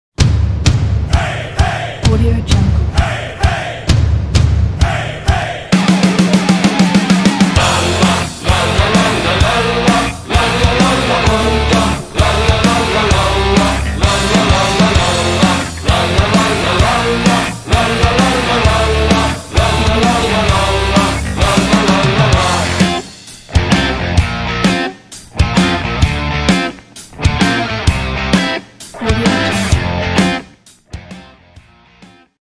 Oktoberfest Titles
This template showcases 8 Oktoberfest titles in a bright and colorful neon style. There are many controls and customizations to make your animation unique. The template has a stunning design and stylish 2d and 3d elements. Edit the text and hit render!